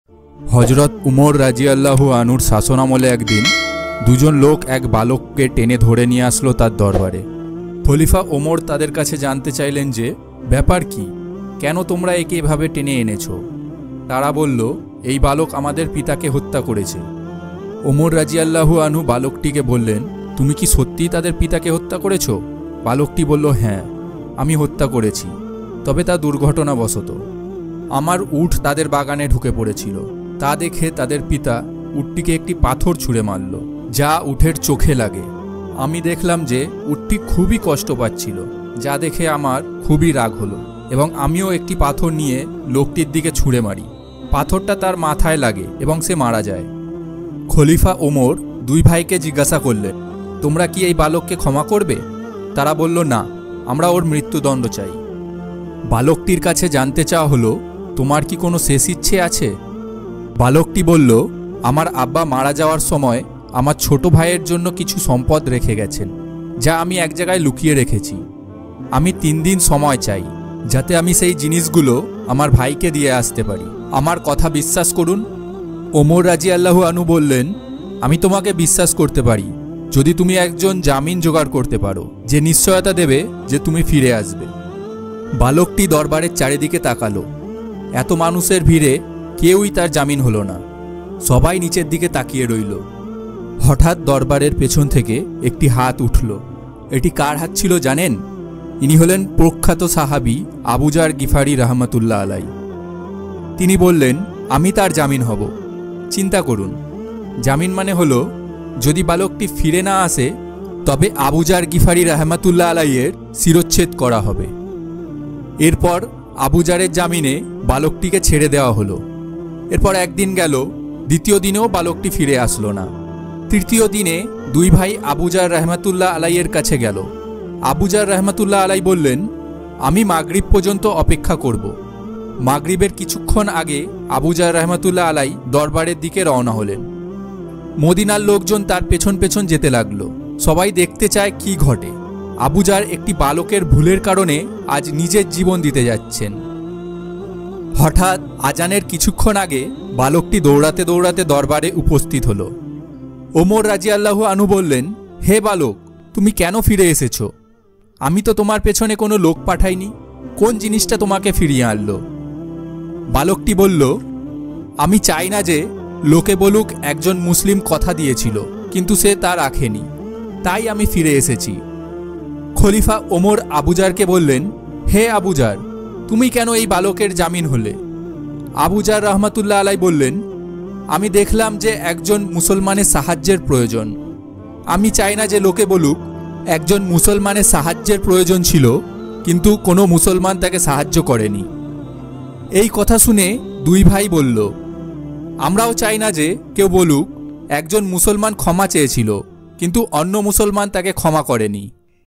হজরত ওমর (রাঃ)-এর শাসন আমলে একজন হত্যাকারী যুবক ও আবুযার গিফারী (রাঃ) এর ঘটনা।
একজন হত্যাকারী যুবক ও আবুযার গিফারী (রাঃ) এর ঘটনা।

হজরত_ওমর(রাঃ)
আবুযার_গিফারী(রাঃ)